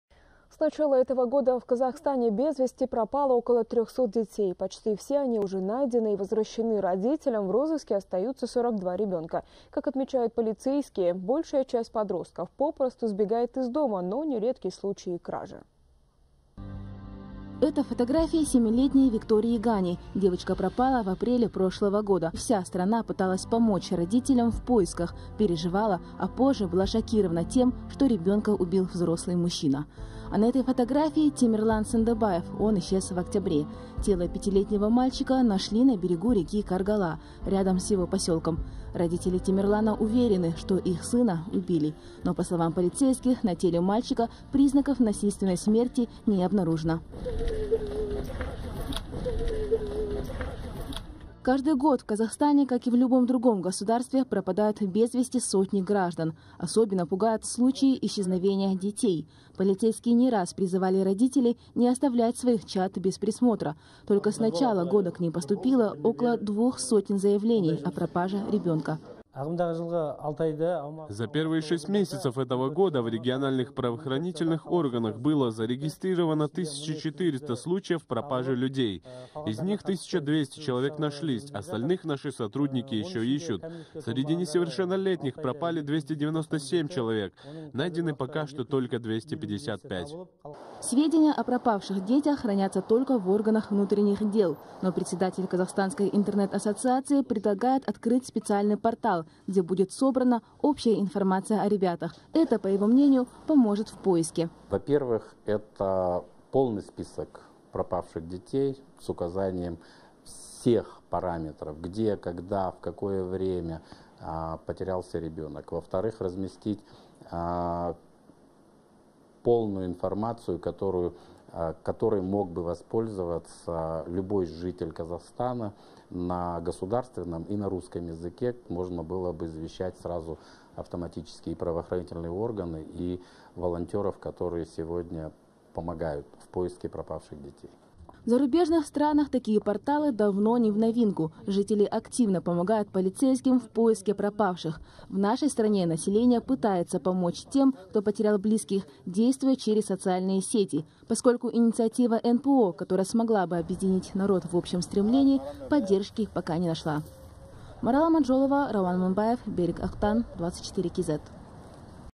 С начала 2015 года в Казахстане без вести пропало около 300 детей
Почти все они уже найдены и возвращены родителям. В розыске остаются 42 ребенка.